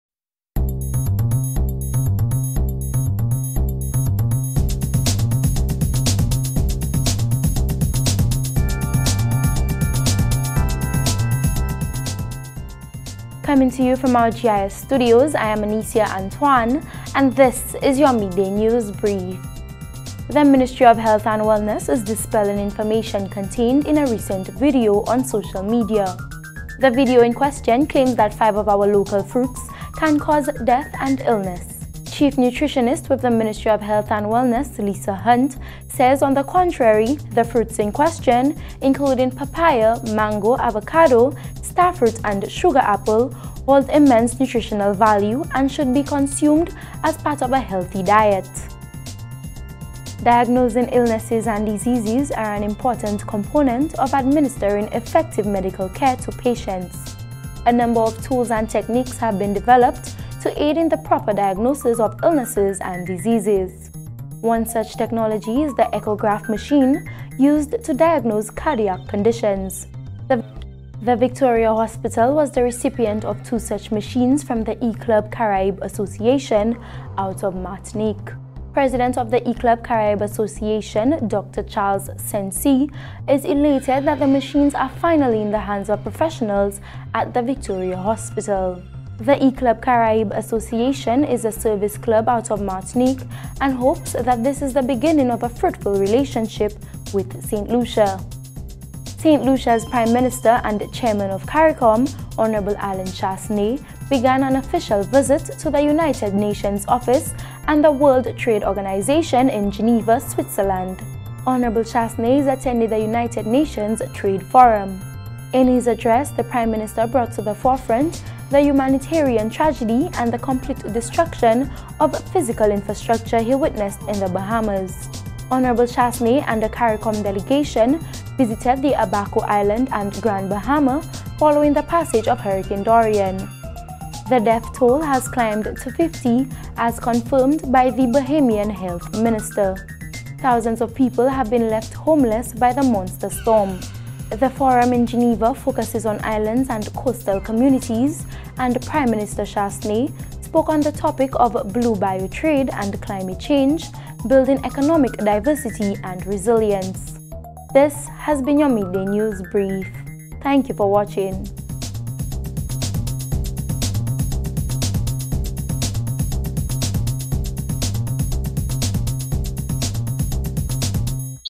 GIS Midday News Brief (Sep. 13, 2019)
In today’s news brief:
The Ministry of Health and Wellness dispels misinformation focusing on five local fruits; Victoria Hospital receives echograph machine donation; and Prime Minister Allen Chastanet visits the UN in Geneva, Switzerland.